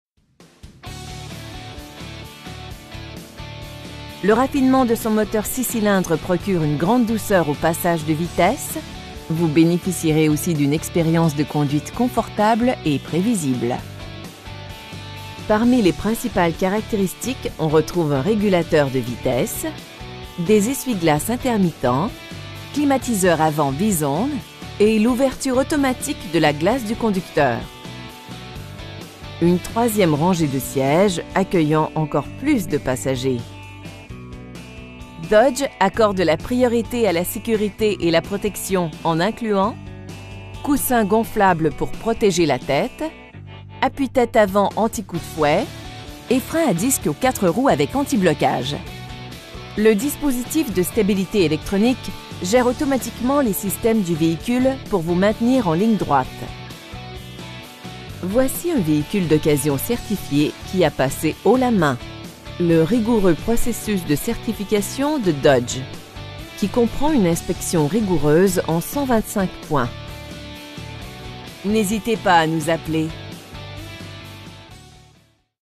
2014 Dodge Grand Caravan SXT 7 PASS STOW GROUPE ELECTRIQUE
Montez à bord et laissez-vous éblouir par la Dodge Grand Caravan 2014. Le raffinement de son moteur 6 cylindres procure une grande douceur aux passages des vitesses, vous bénéficierez aussi d'une expérience de conduite confortable et prévisible. Parmi les principales caractéristiques on retrouve un régulateur de vitesse, des phares à extinction temporisée, un compte-tours, des essuie-glaces intermittents, un ordinateur de bord, un afficheur de la température extérieure, climatiseur avant bizone, et l'ouverture automatique de la glace du conducteur. L'habitacle propose de nombreuses solutions de rangement qui sont le reflet d'un souci élevé du détail. Une 3e rangée de sièges accueillant encore plus de passagers. Dodge accorde la priorité à la sécurité et la protection en incluant: coussins gonflables à l'avant avec détecteur de poids du passager, rideaux gonflables pour protéger la tête, système antipatinage, système d'assistance au freinage, appuis-tête avant anti-coup de fouet, la mise hors de fonction du système d'allumage, et freins à disque aux 4 roues avec antiblocage. Le dispositif de stabilité électronique gère automatiquement les systèmes du véhicule pour vous maintenir en ligne droite. Voici un véhicule d'occasion certifié qui a passé haut la main le rigoureux processus de certification de Dodge, qui comprend une inspection rigoureuse en 125 points! Il n'y a qu'une chose qui nous satisfait: surpasser les attentes de chacun de nos clients. N'hésitez pas à nous appeler.